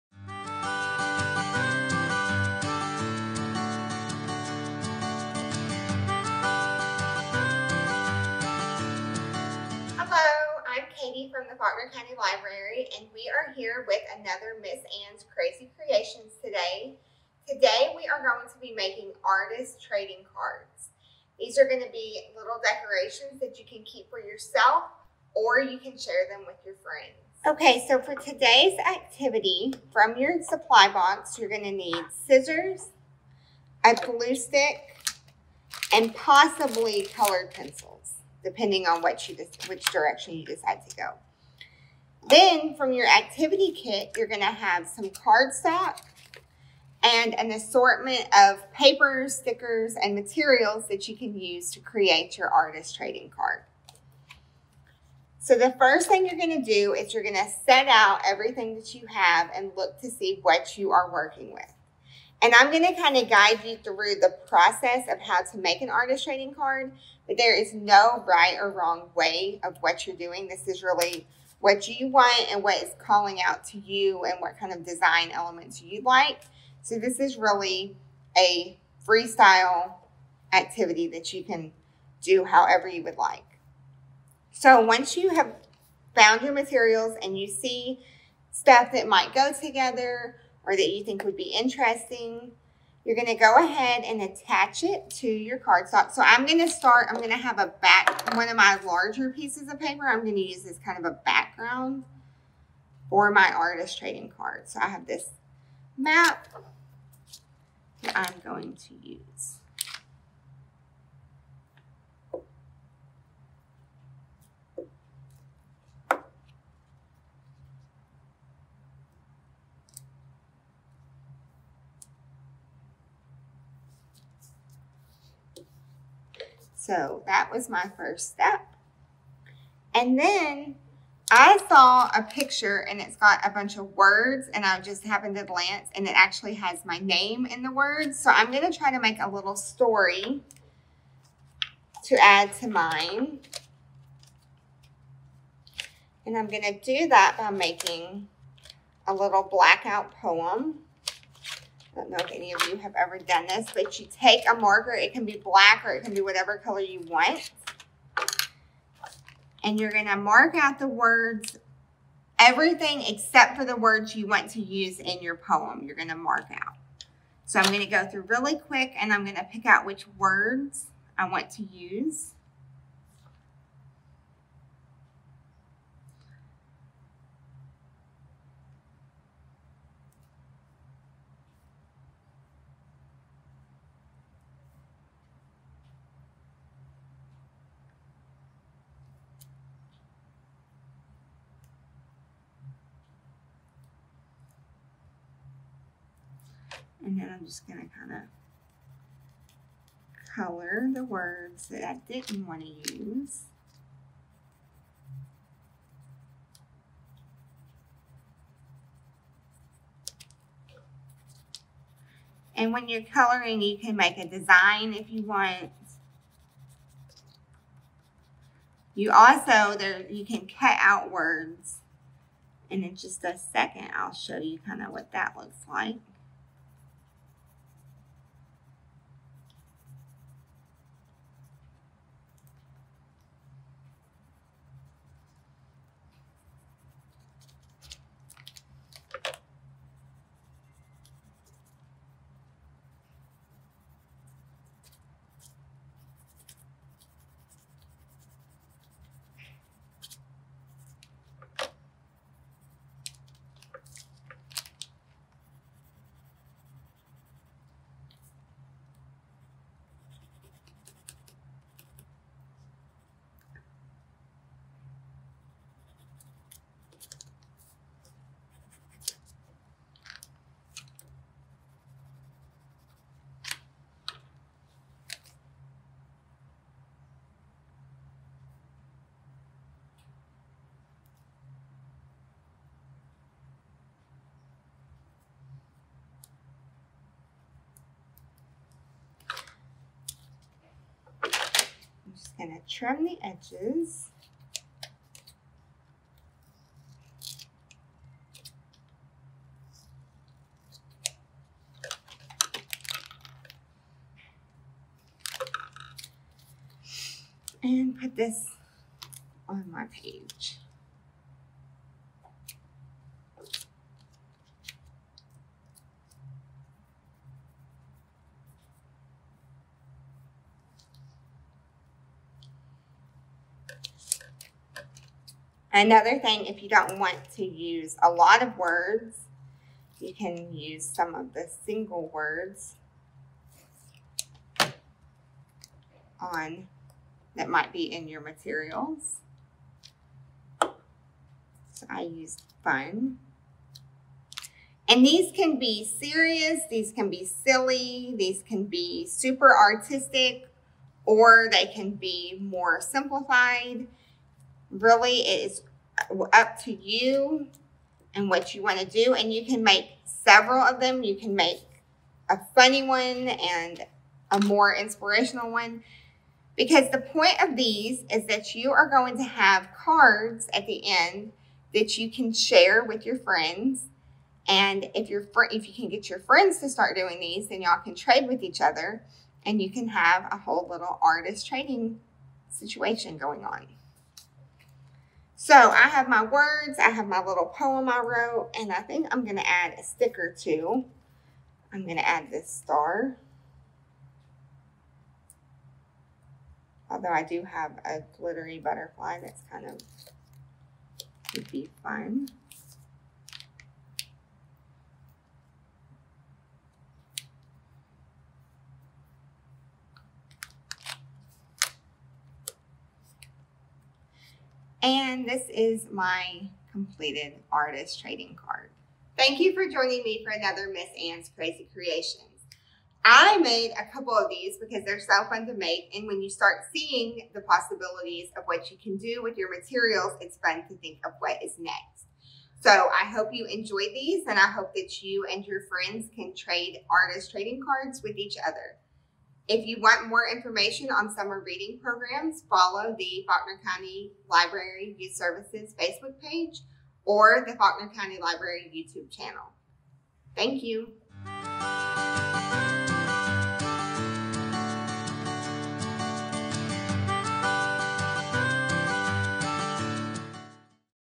Ms. Ann's Crazy Creations with Ms. Katie (Artist Trading Card)
Join Ms. Katie as we make Artist Trading Cards.